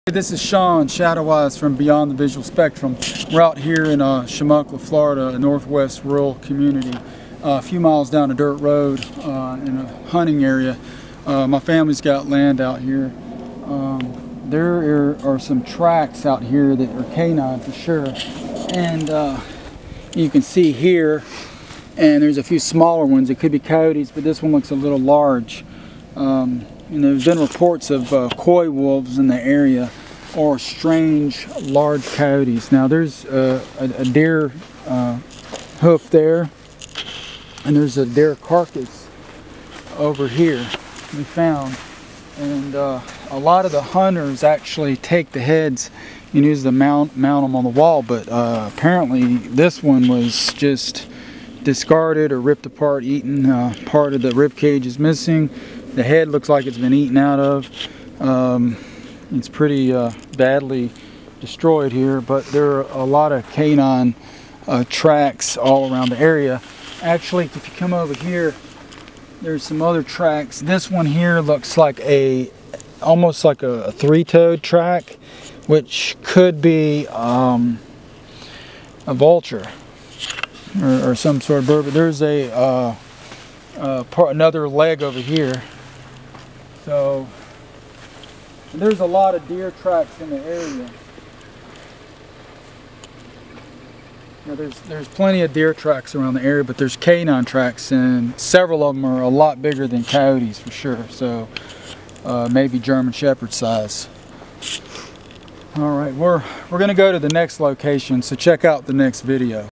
Deer carcass found & strange looking large coyotes seen nearby!!
Investigating a deer carcass a few miles down a Florida dirt road after recent reports of extremely large coyotes were seen near this area.